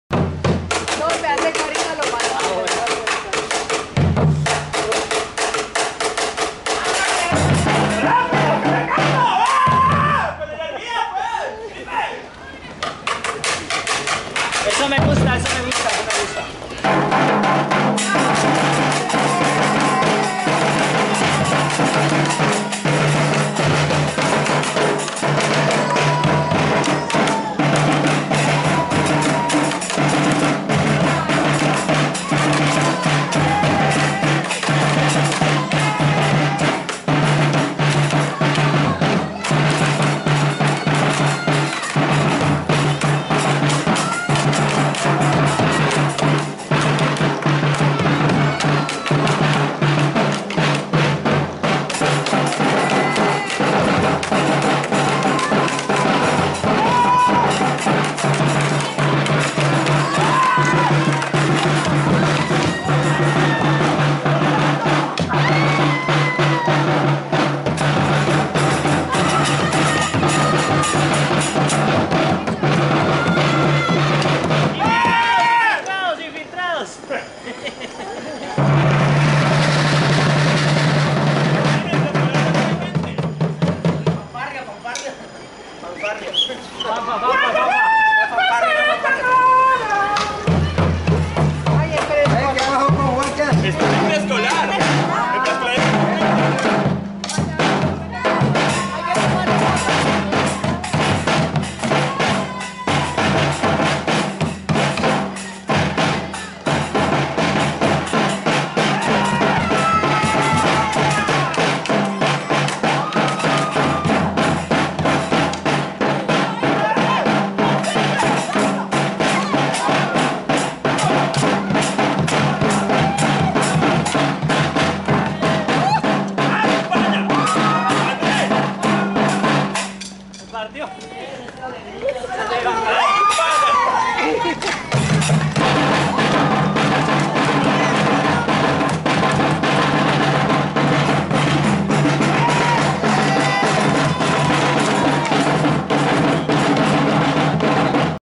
Recibimiento en Primaria - Colegio Claret
En el día del estudiante, los profesores de primaria prepararon un recibimiento muy especial para sus estudiantes...